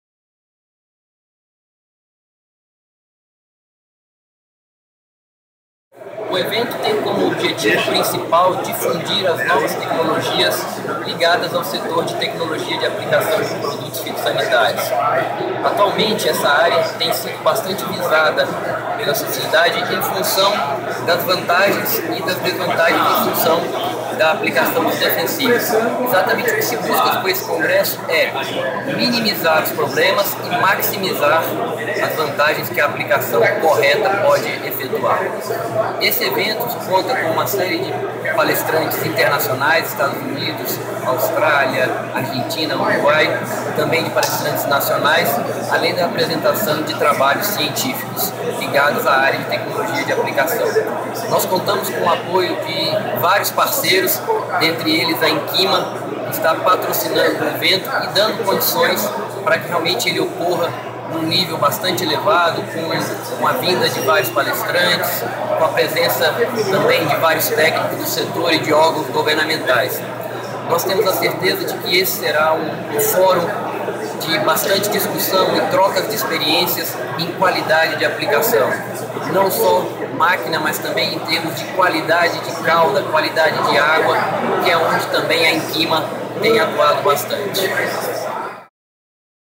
VII SINTAG - Simpósio Internacional de Tecnologia de Aplicação
Um dos principais eventos da área de Tecnologia da Aplicação está acontecendo entre os dias 14 e 16 de Setembro, em Uberlândia (MG). Trata-se do VII Sintag – Simpósio Internacional de Tecnologia de Aplicação. Confira essa entrevista com o Professor João Paulo Arantes, coordenador do evento, sobre o simpósio e o uso da tecnologia nas aplicações. Informações